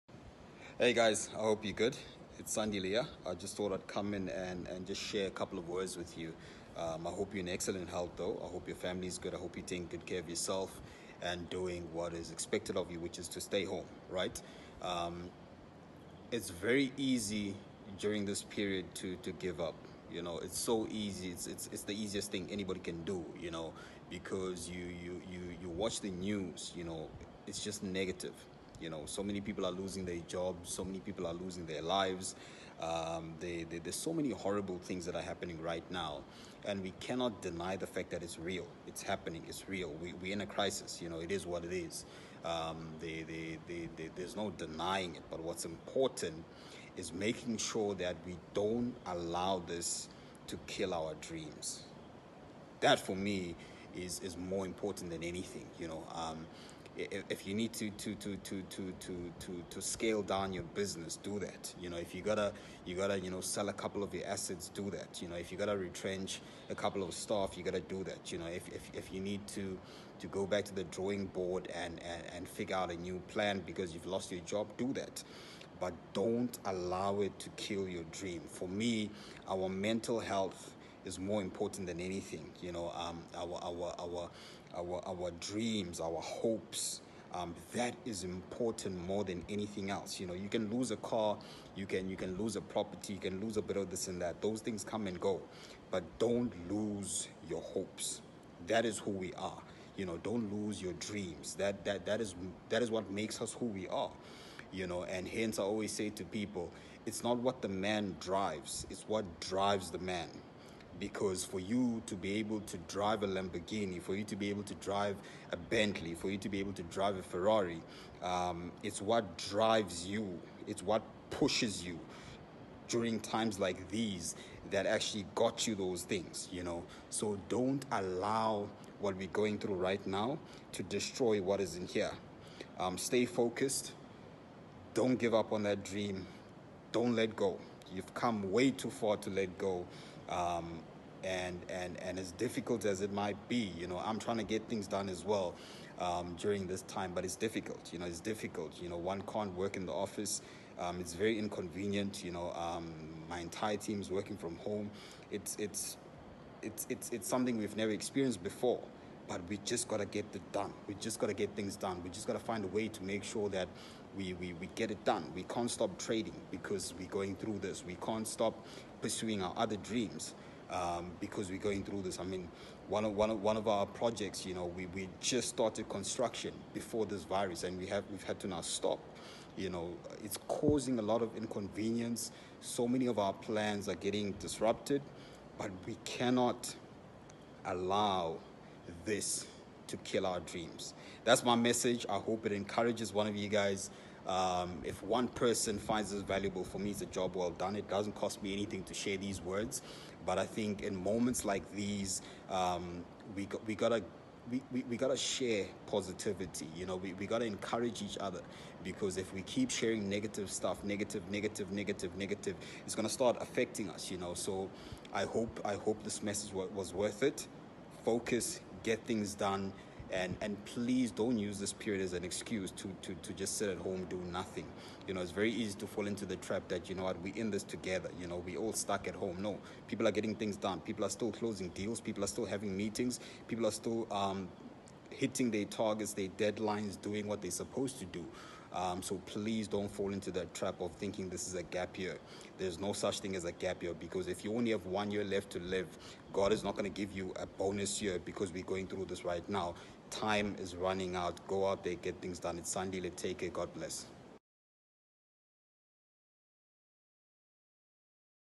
Sandile Shezi: "Don't Let This Virus Kill Your Dreams" || Forex Motivation
In hard and trying times as this, do not loose hope on your dreams.